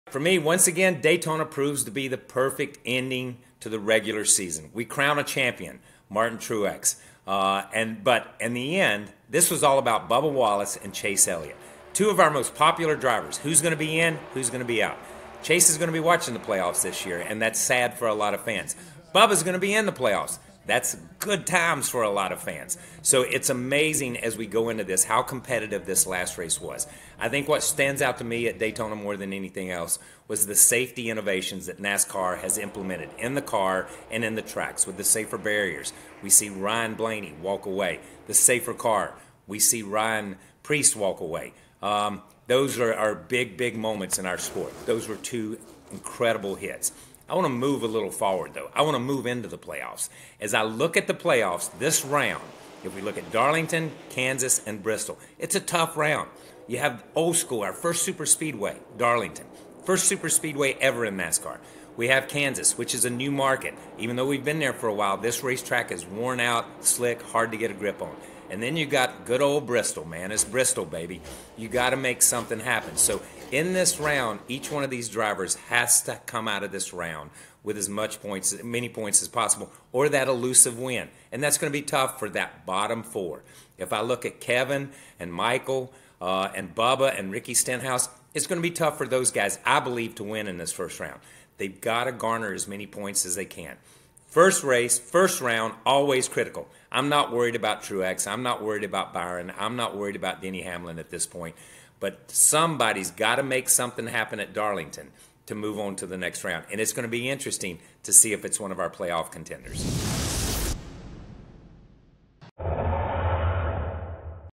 Kyle Petty gives his Round of 16 playoff outlook heading into Darlington | NASCAR
NBC's Kyle Petty dives into the Round of 16 as the NASCAR Cup Series heads to Darlington for the first race of the NASCAR Playoffs.

Play every weekend!